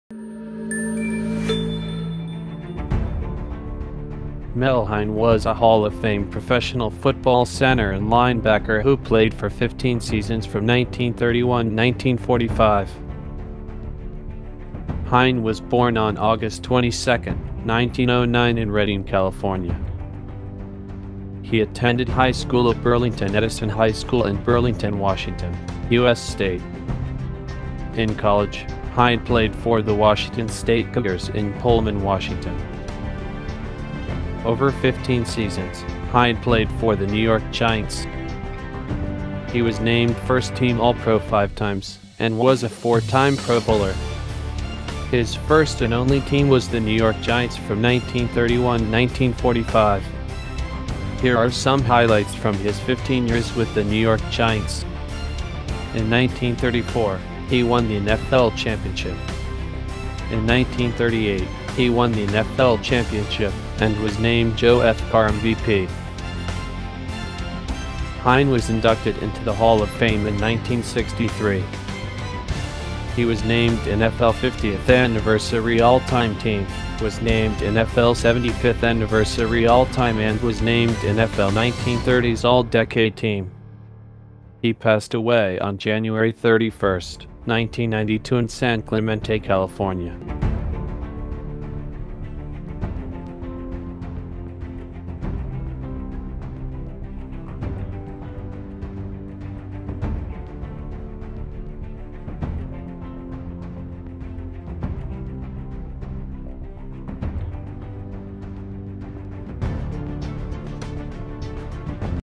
Mel Hein: Hall of Fame Football Center and Linebacker
Mel Hein was a Hall of Fame professional football center and linebacker who played for 15 seasons from 1931 to 1945.

In college, Hein played for the Washington State Cougars in Pullman Washington.

Professionally, Hein played for the New York Giants.

In 1934, he won the NFL Championship.
In 1938, he won the NFL Championship, and was named Joe F. Carr MVP.

Hein was inducted into the Hall of Fame in 1963.

He was named NFL 50th Anniversary All-Time Team, was named NFL 75th Anniversary All-Time and was named NFL 1930s All-Decade Team.